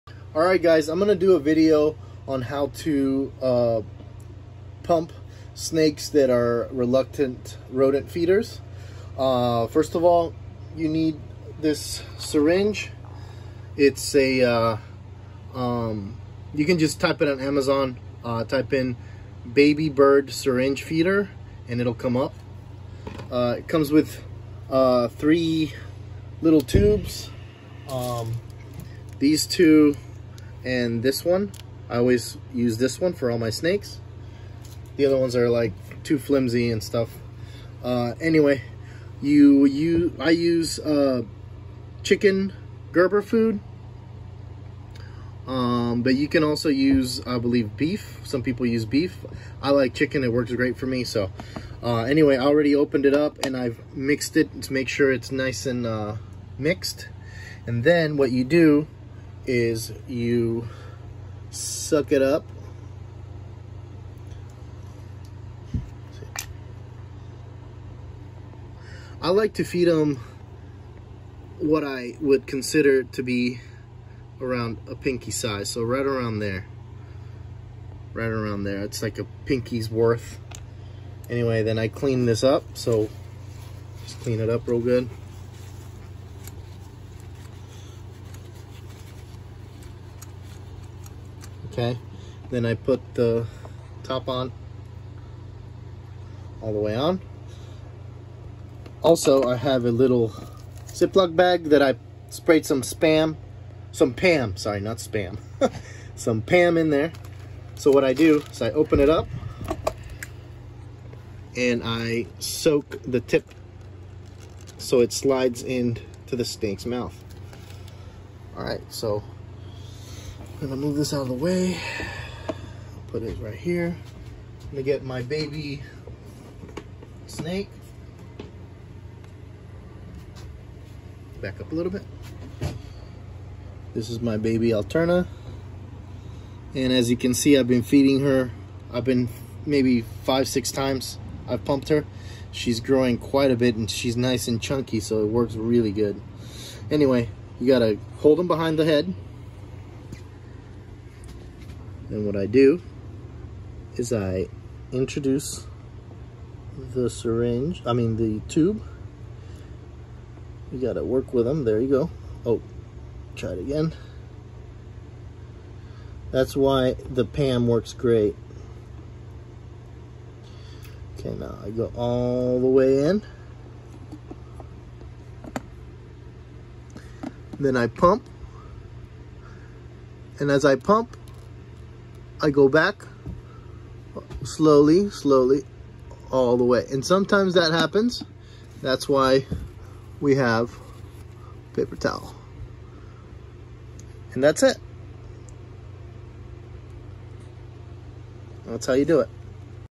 Tube feeding snakes!! How to keep reluctant rodent feeders nice and healthy.
Instructions on how to tube syringe feed snakes.